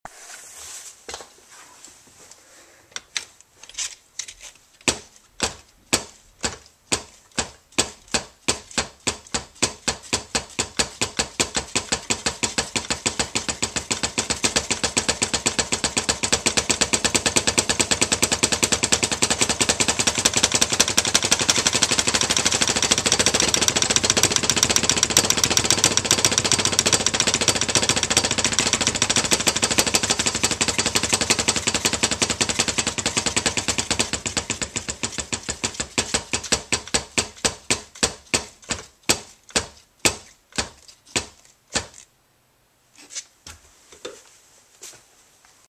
heel up/floating double bass technique demo
This is me demonstrating my bass drum technique with an open-closed-open single stroke roll going up to near (but still less than) as fast as i can play. the metronome marking would be somewhere around 210 bpm assuming those are 16ths. I have been known to hit 220 on a good, warmed up day.

at no point in this video am i playing heel down.  its all heel up or floating technique.   this is on a 2000 or 2001 model iron cobra power glide with high but not maximum spring tension and beaters about 6 inches from the head.

here is an explanation.

yes, i could have (should have) played this more smoothly but i am too lazy to do another take.  i know its not world class speed or anything, and i am not trying to say that its anything special, but its quick enough for most situations.